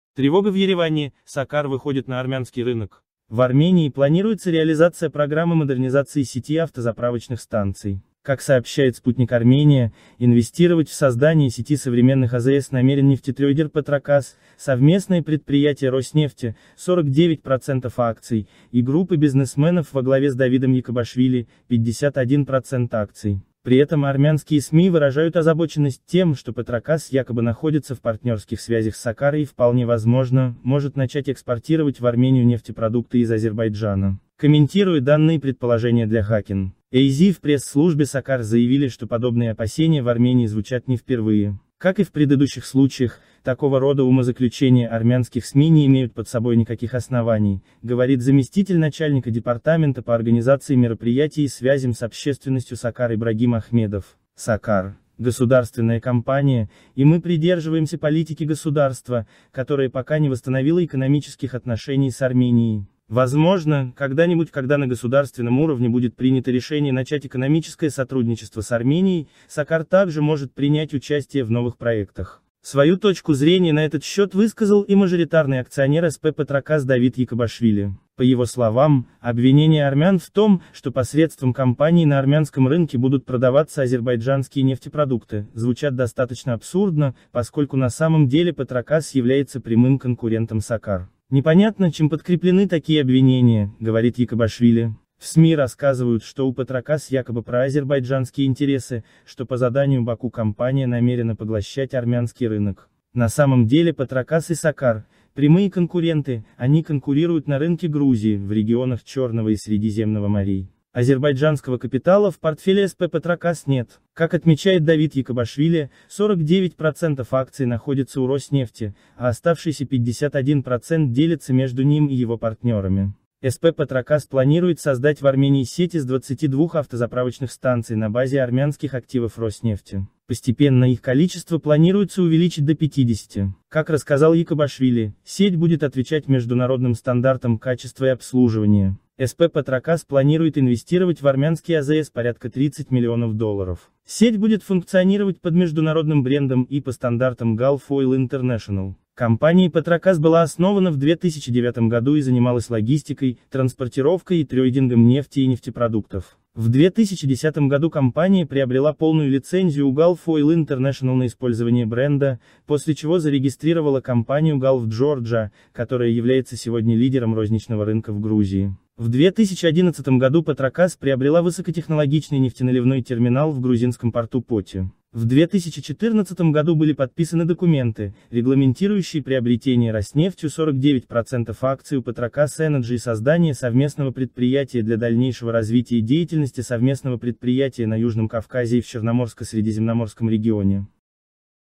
Тревога в Ереване! SOCAR выходит на Армянский рынок?
В Армении планируется реализация программы модернизации сети автозаправочных станций. Как сообщает «Спутник Армения», инвестировать в создание сети современных АЗС намерен нефтетрейдер Petrocas - совместное предприятие «Роснефти» (49% акций) и группы бизнесменов во главе с Давидом Якобашвили (51% акций). При этом армянские СМИ выражают озабоченность тем, что «Petrocas якобы находится в партнерских связях с SOCAR и, вполне возможно, может начать экспортировать в Армению нефтепродукты из Азербайджана».